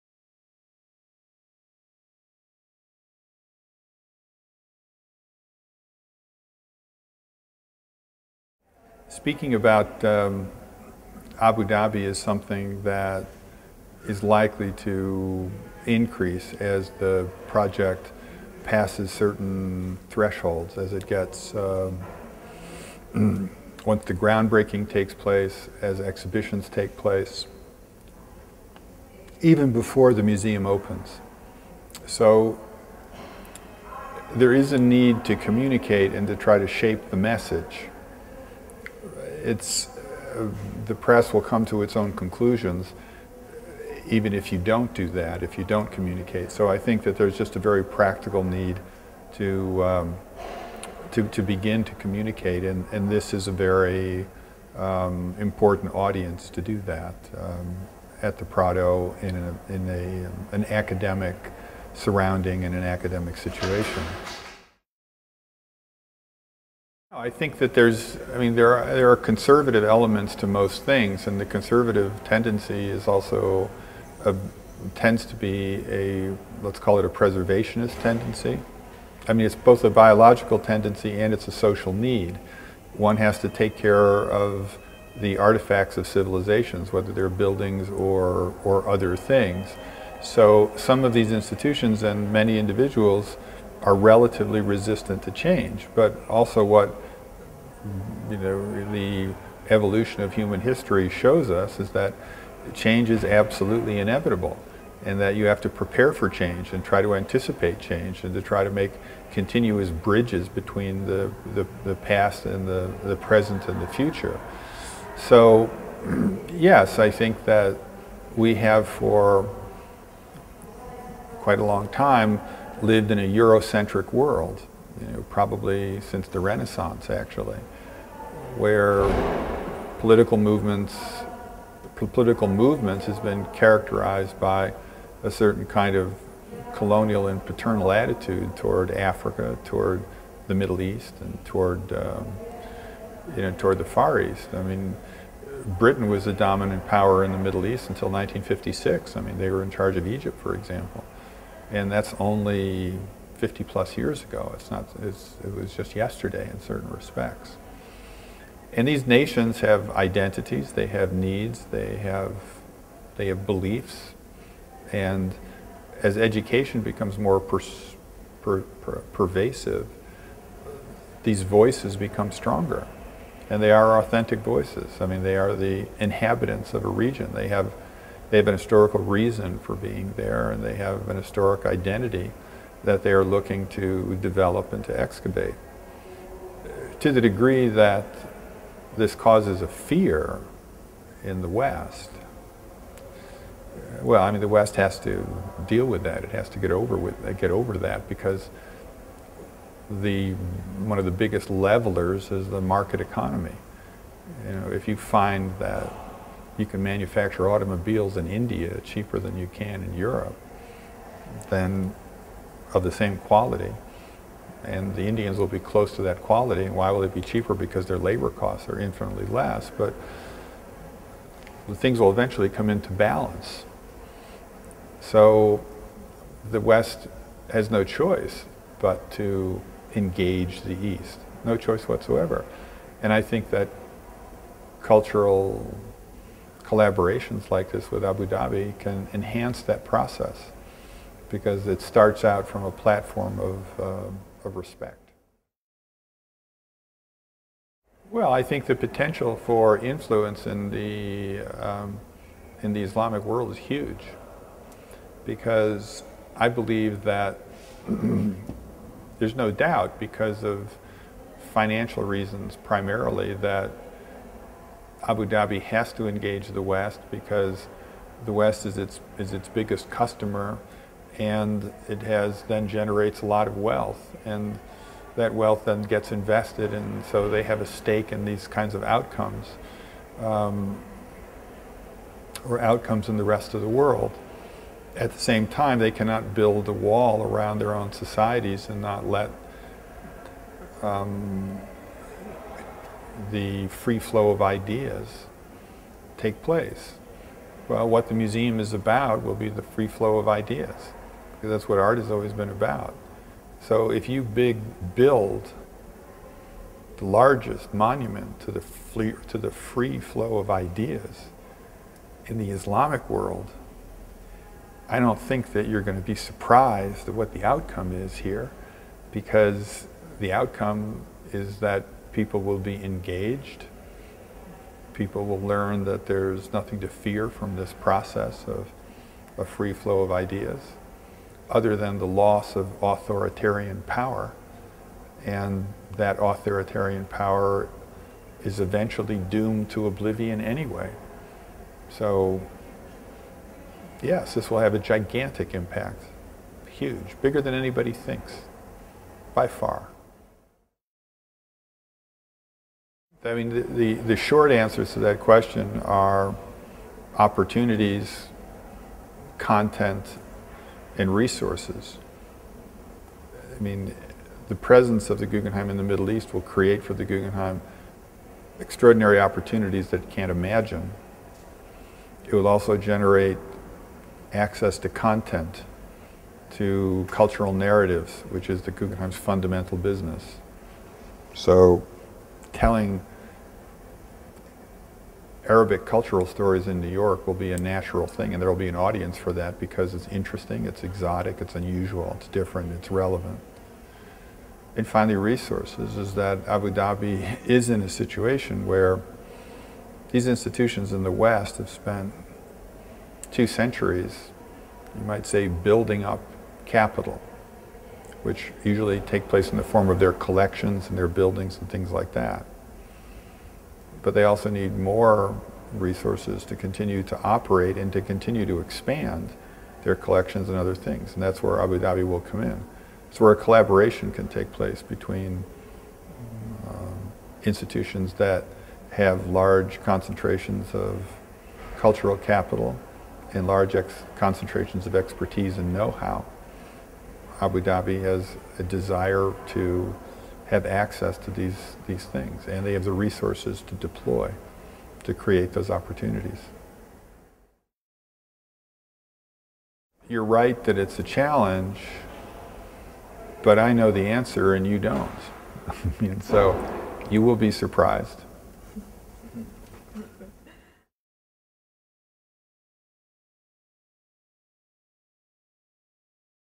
Entrevista a Thomas Krens: La necesidad de comunicar
Entrevista al Senior Advisor for International Affairs, The Solomon R. Guggenheim Foundation, Thomas Krens con motivo de la conferencia "El nuevo proyecto de Abu Dhabi" del Curso de Verano "El museo real, imaginario y virtual La enseñanza del arte, nuevos métodos y técnicas" celebrado en el Museo del Prado en julio 2009.